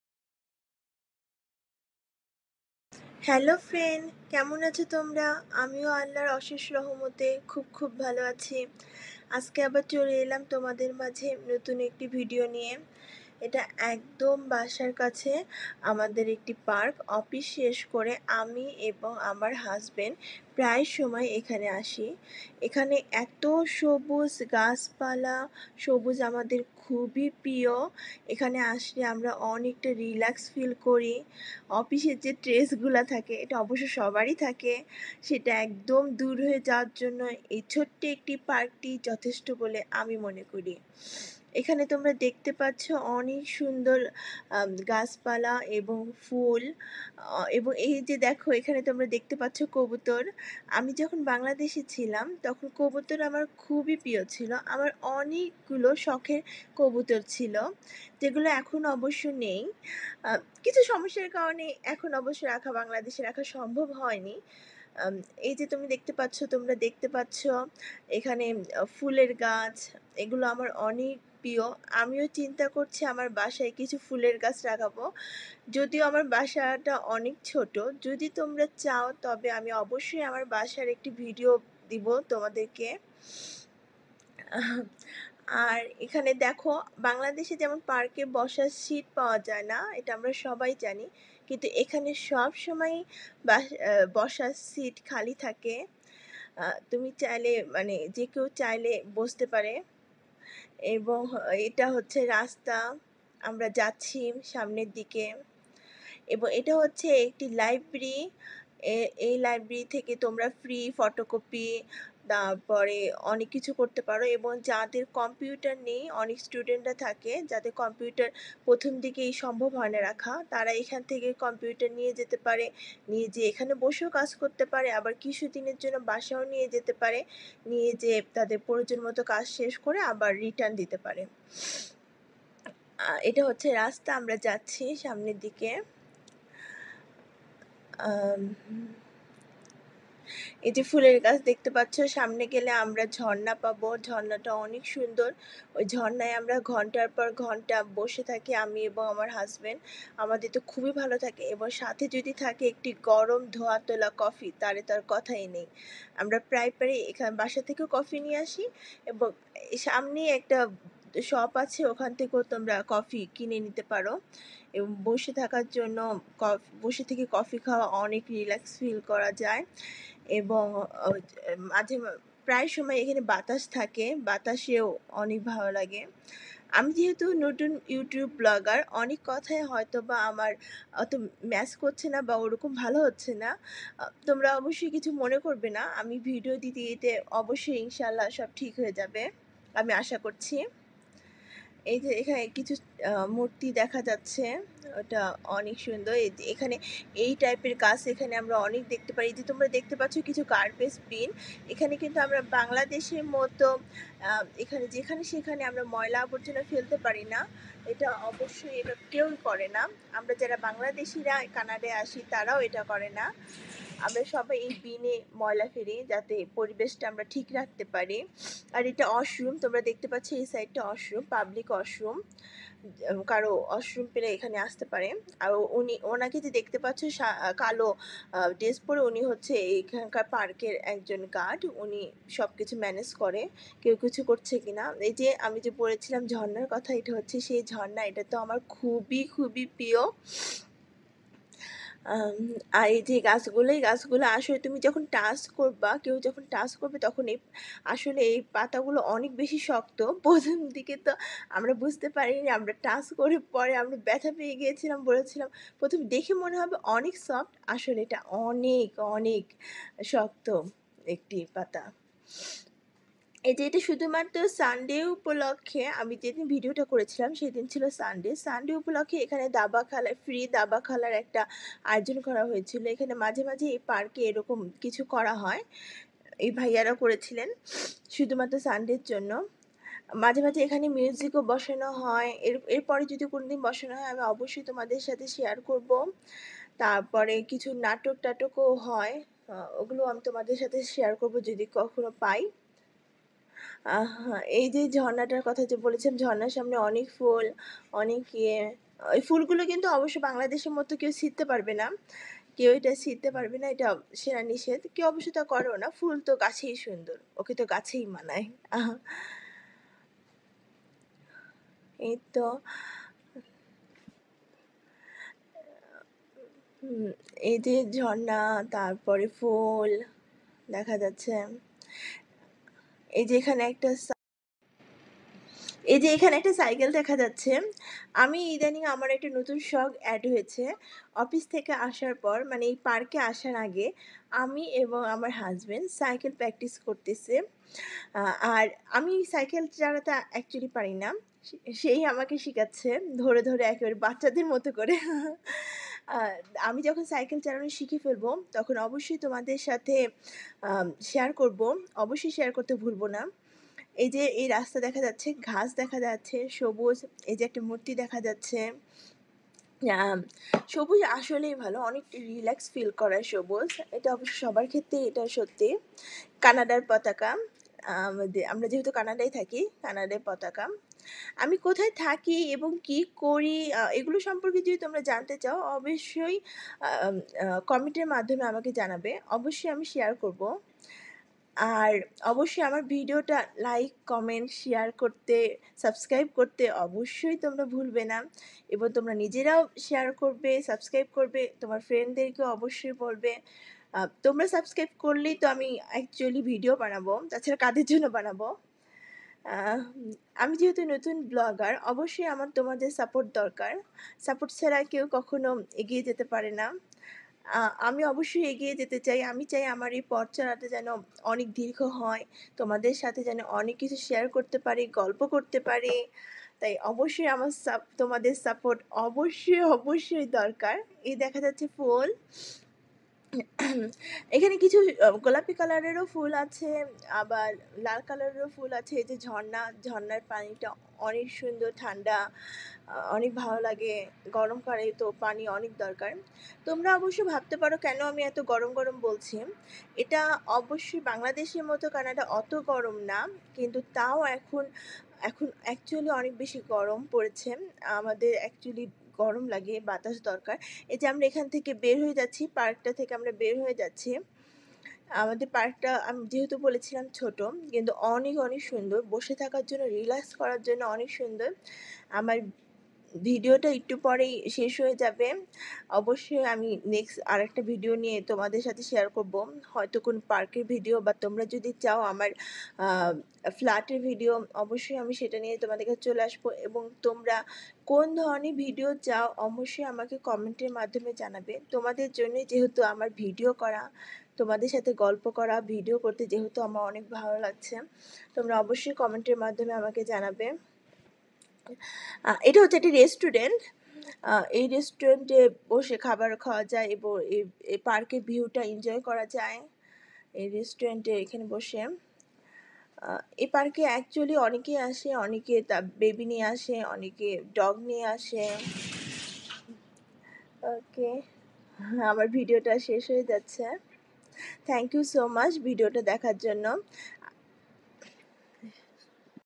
Exploring the Serene Beauty of Memorial Park Library and Garden | Downtown Calgary, Canada
Join us on a captivating journey through the serene Memorial Park Library and Garden in Downtown Calgary, Canada. Immerse yourself in the enchanting beauty of this oasis, with a mesmerizing Bangla voice-over specially crafted for our Bangladeshi viewers. Explore hidden gems, lush greenery, and be transported to a world of tranquility and inspiration.

Translation:
কানাডার ডাউনটাউন ক্যালগারিতে নির্মল মেমোরিয়াল পার্ক লাইব্রেরি এবং গার্ডেনের মাধ্যমে একটি মনোমুগ্ধকর যাত্রায় আমাদের সাথে যোগ দিন। এই মরূদ্যানের মনোমুগ্ধকর সৌন্দর্যে নিজেকে নিমজ্জিত করুন, আমাদের বাংলাদেশী দর্শকদের জন্য বিশেষভাবে তৈরি করা একটি মন্ত্রমুগ্ধ বাংলা ভয়েস-ওভার সহ। লুকানো রত্ন, সবুজ সবুজাভ অন্বেষণ করুন এবং প্রশান্তি ও অনুপ্রেরণার জগতে নিয়ে যান।